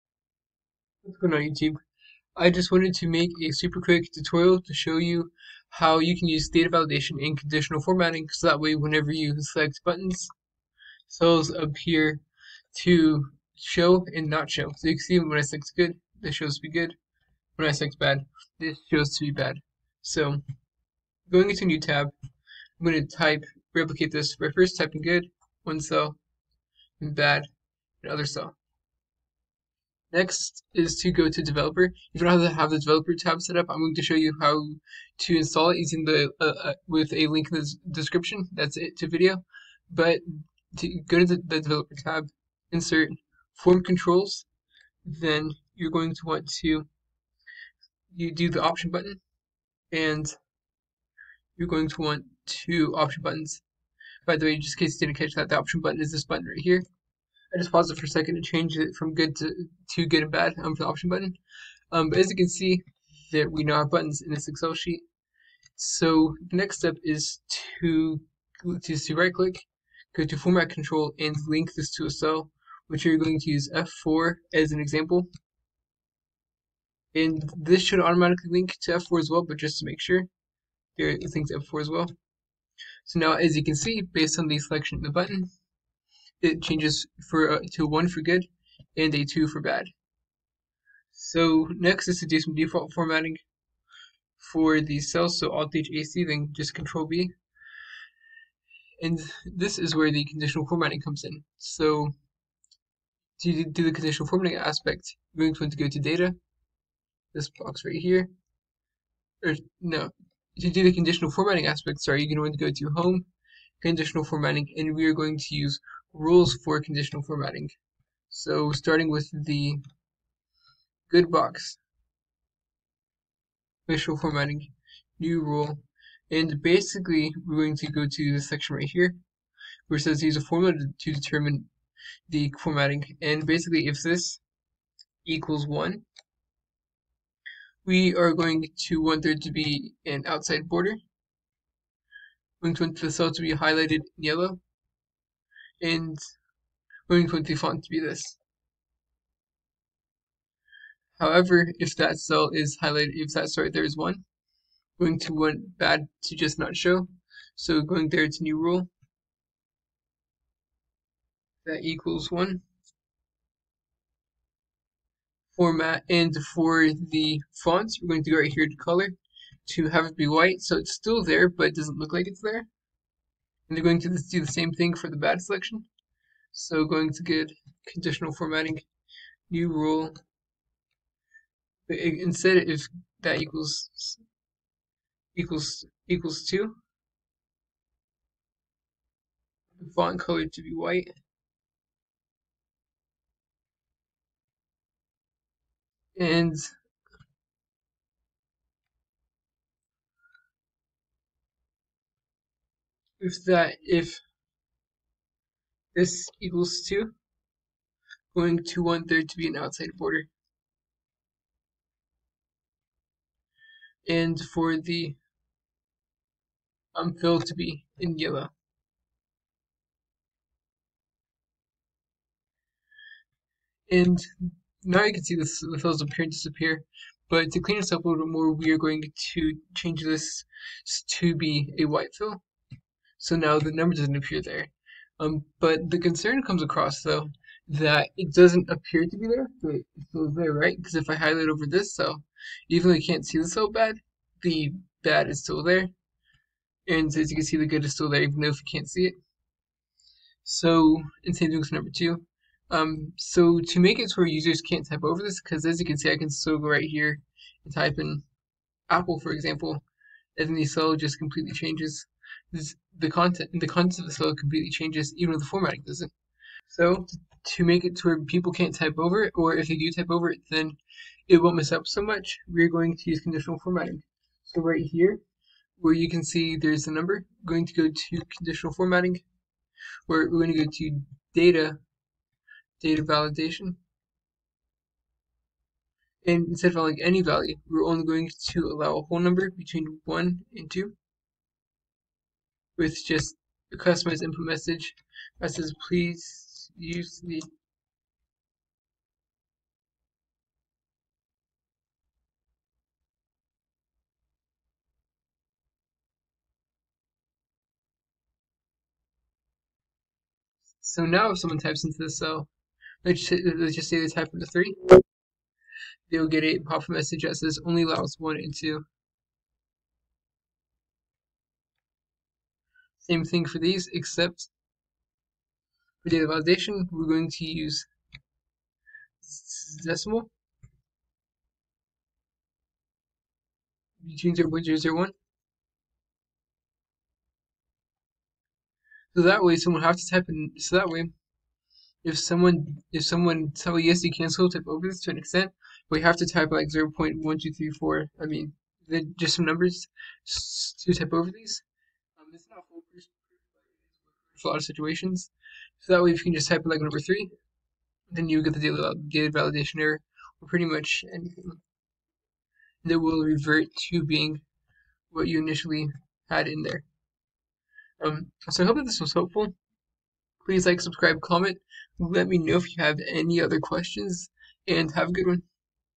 How to Make Cells Appear and Disappear Based On Buttons in Excel.
Hows it going Youtube? I just wanted to make a video to show you how you can make cells appear and disappear based on buttons that you select! This is very useful if you want to make an interactive interface in excel, and bring your sheets and presentations to the next level!! This is an awesome tool for dashboards, and is great because it does not require any macros or VBA, and uses data validation and conditional formatting instead. Have a great day!!

Get a FREE share of stock with Robinhood!!

An Excel Cheat Sheet

Table of Contents. No Formula Needed With an Easy Home Link!"